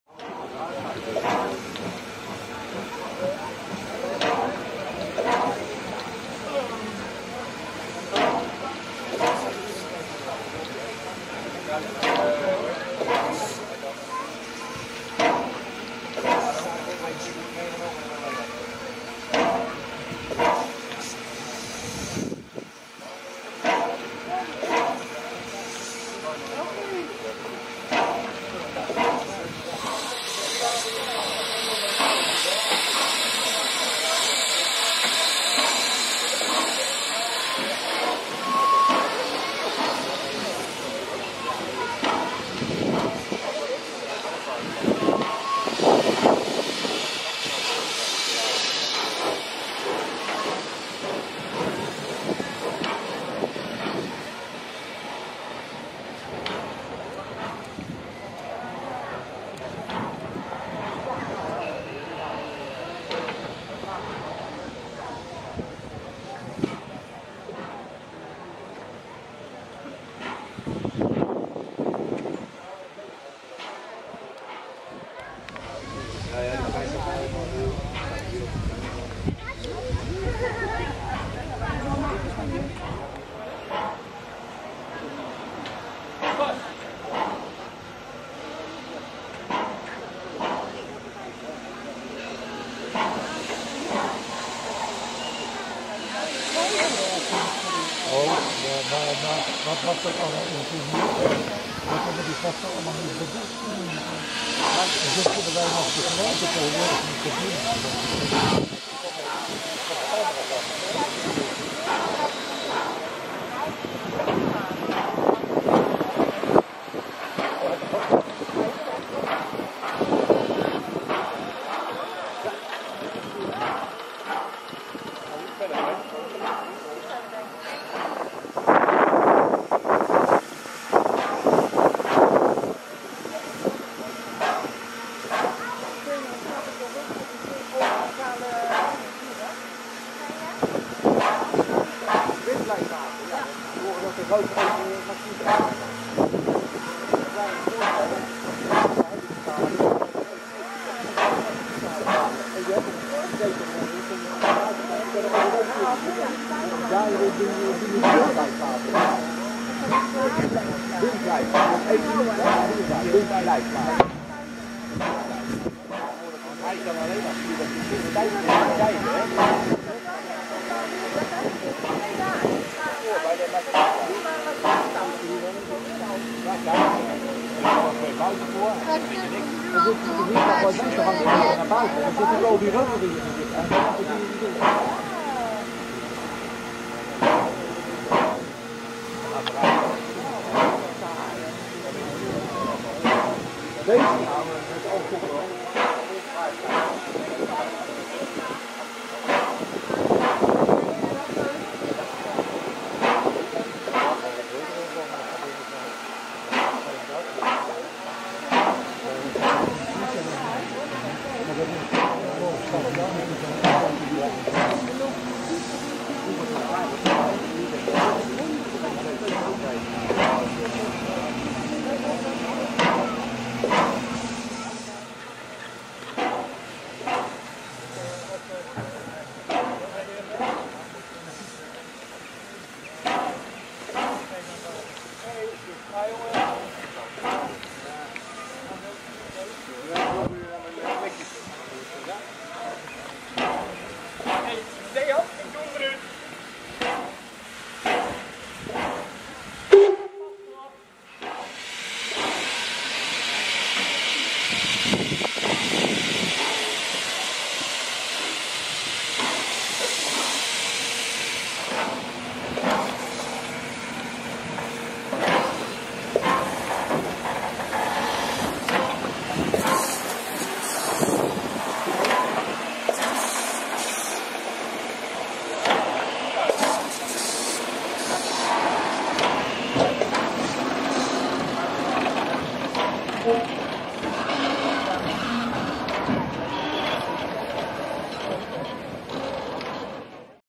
SSN 01 1075 in Beekbergen op bezoek bij de Veluwsche Stoomtrein Maatschappij
Op 4 juni 2017 is de stoomlok 01.1075 van de Stoom Stichting Nederland op bezoek bij de Veluwsche Stoomtrein Maatschappij in Beekbergen.
De Stoomlok wordt daar gerestaureerd (van water en kolen voorzien). Daarna gekeerd op de draaischijf en 16:00 uur wordt er weer vertrokken voor de treinreis met 2 rijtuigen via Dieren, Arnhem, Den Bosch en Dordrecht naar Rotterdam. Het betreft een besloten rit.